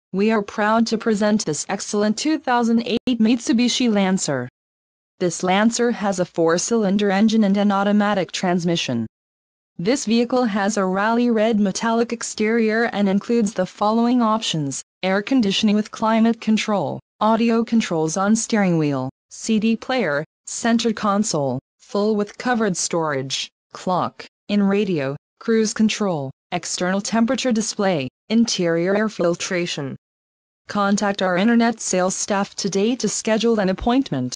2008 Mitsubishi Lancer Gulfport MS
Allen Toyota Scion Hyundai
624 E. Pass Rd.

2008 Mitsubishi Lancer GTS
Engine: L4, 2.0L
Trans: Automatic
Color: Rally Red Metallic
Mileage: 29,792